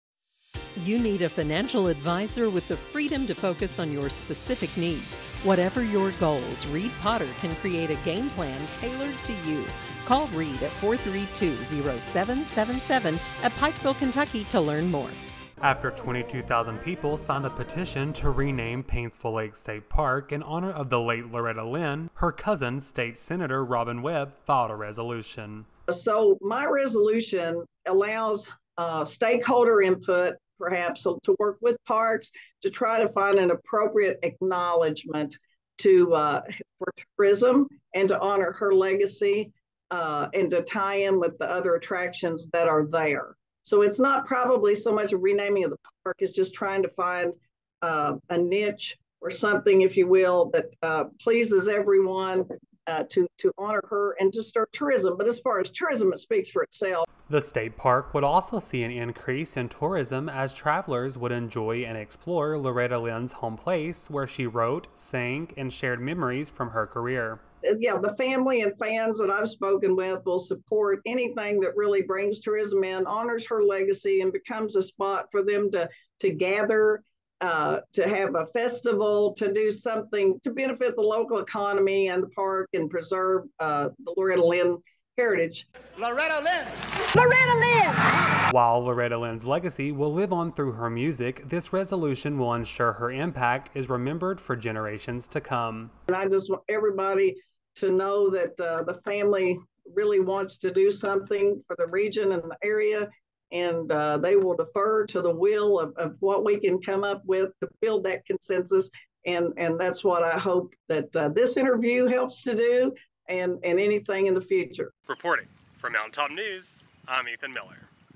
Resolution Filed to Rename Paintsville Lake State Park After Loretta Lynn
A resolution has been filed to rename Paintsville Lake State Park after the late Loretta Lynn.